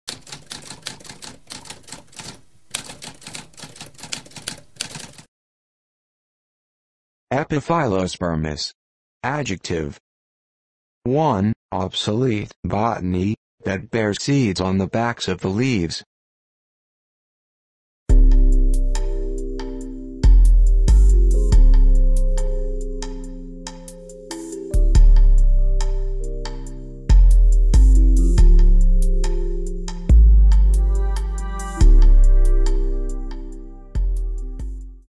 What does epiphyllospermous mean?
A spoken definition of epiphyllospermous.

Intro Sound:
Typewriter - Tamskp